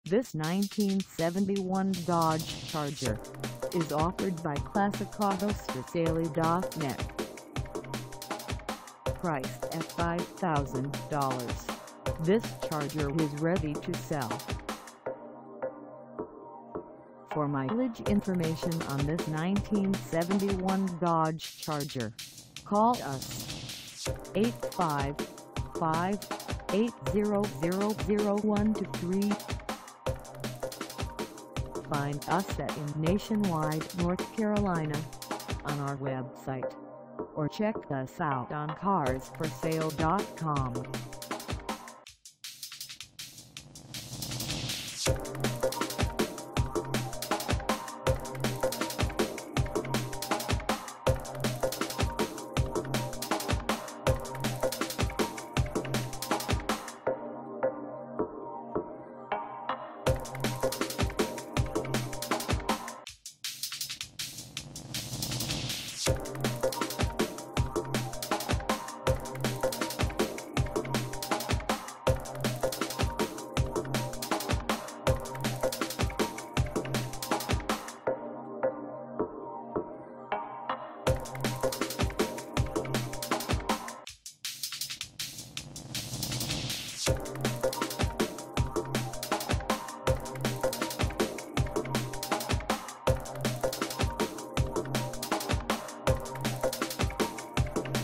1971 Dodge Charger 500 for sale in Nationwide, NC 27603 at C
This 1971 Dodge Charger 500 is for sale in Nationwide, NC 27603 at Classicautosforsale.net.
 392090
Engine: Unspecified 318
Transmission: Automatic
Color: Unspecified
Mileage: 0
Contact Classicautosforsale.net Nationwide, NC 27603 to test drive this 1971 Dodge Charger 500 today. List your classic . The best classic car advertising on the planet!